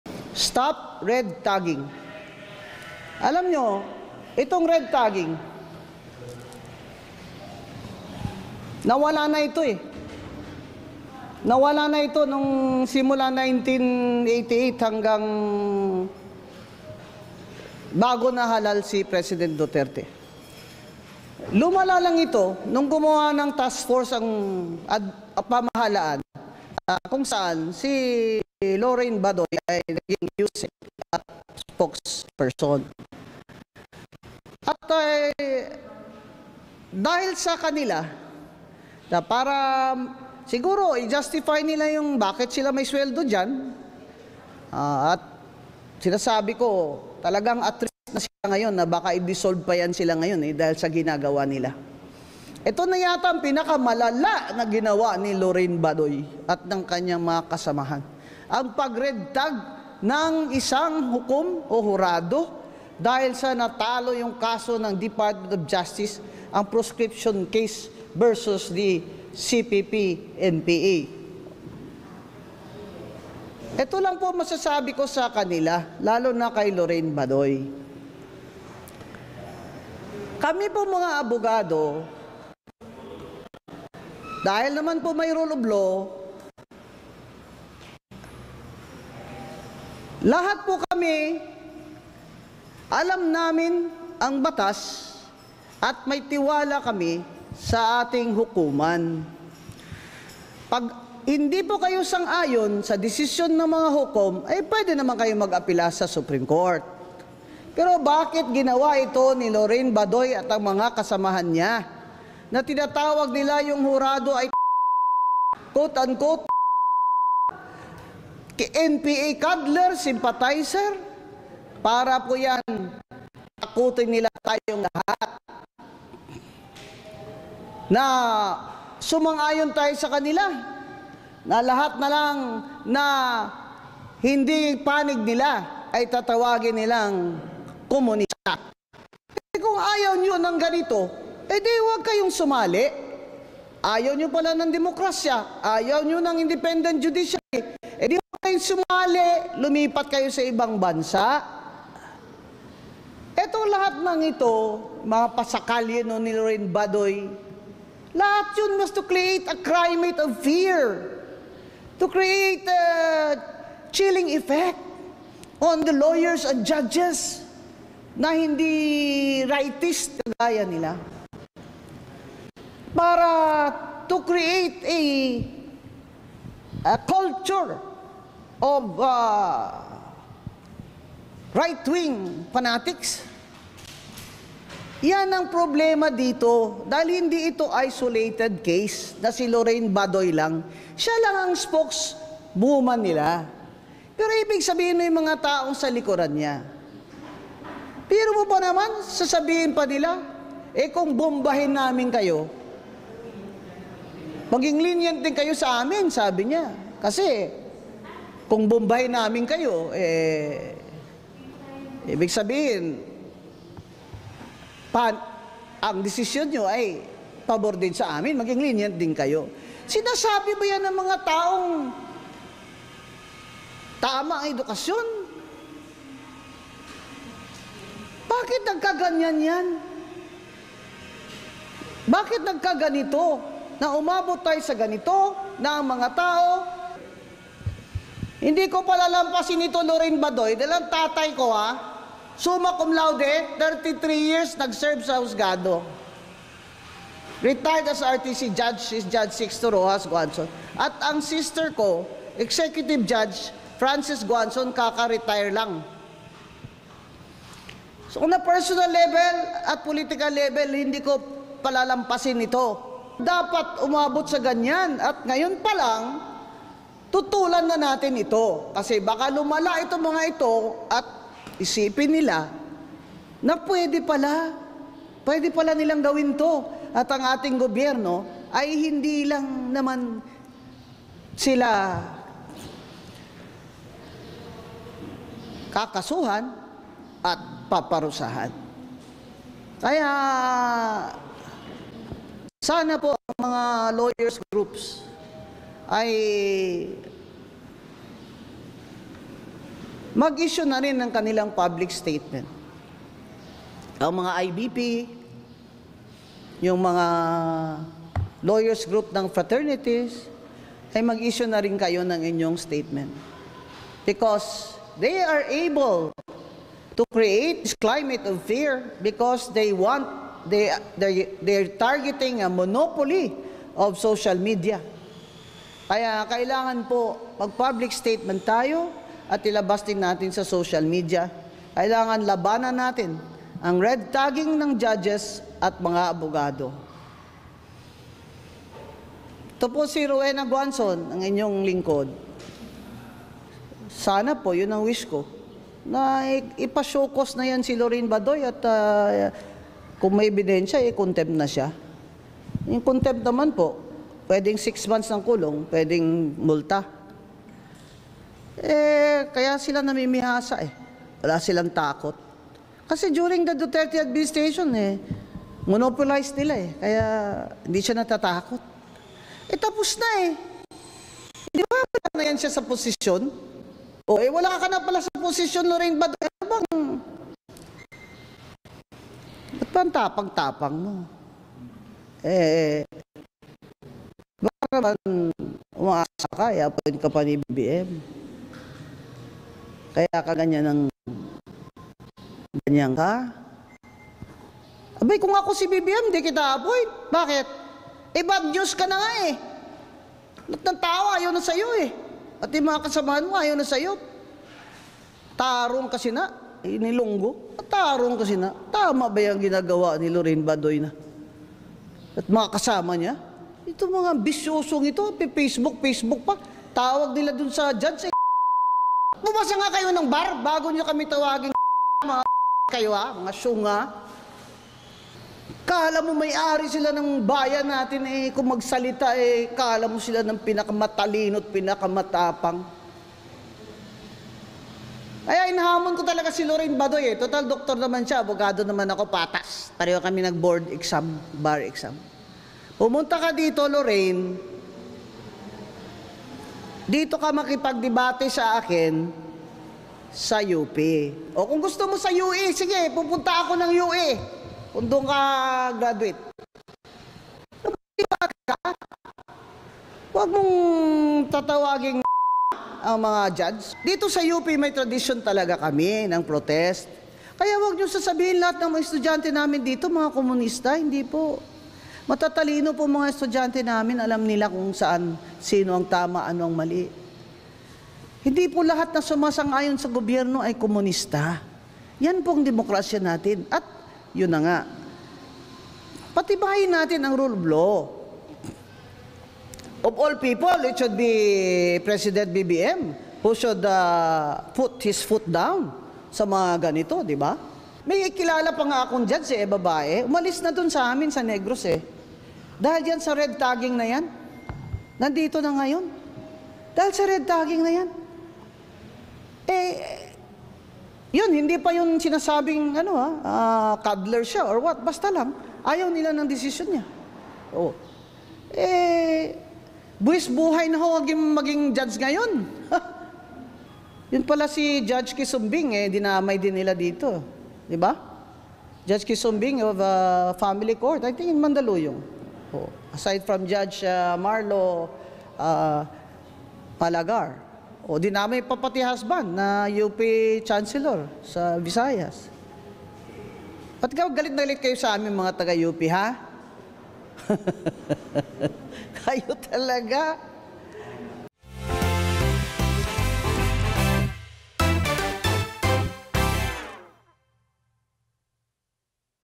Guanzon, hinamon ng debate si Badoy
Hinamon ni dating COMELEC commissioner Rowena Guanzon si dating NTF-ELCAC spokesperson Lorraine Badoy sa isang debate. Kasunod ito ng pag-atake ni Badoy laban kay Manila Regional Trial Court judge Marlo Mardoza-Malagar, ang hukom na nagbasura sa proscription case ng gobyerno na humihiling na maideklarang terorista ang Communist Party of the Philippines-New People's Army (CPP-NPA).

Sinabi ito ni Guanzon sa isang protesta sa UP Diliman laban sa pangre-red-tag sa mga hukom, September 28. | via Marlene Alcaide